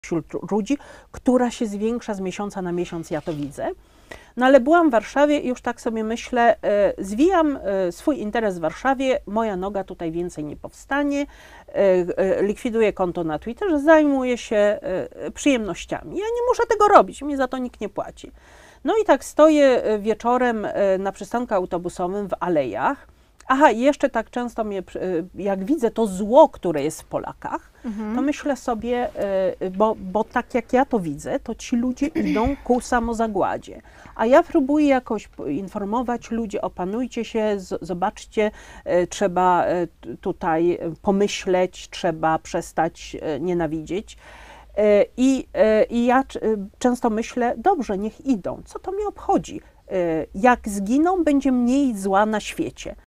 Jak Joanna Teglund Polaków ratowała...
Rękawiczki chronią przed koronawirusem — mówiła Joanna.
Profesorowie są głupi — mówiła Joanna.
Joanna wie lepiej — mówiła Joanna.
Dlaczego Polacy nie panikują? — pytała Joanna.
Powinni panikować — mówiła Joanna.

Jeszcze jakieś pytania?😂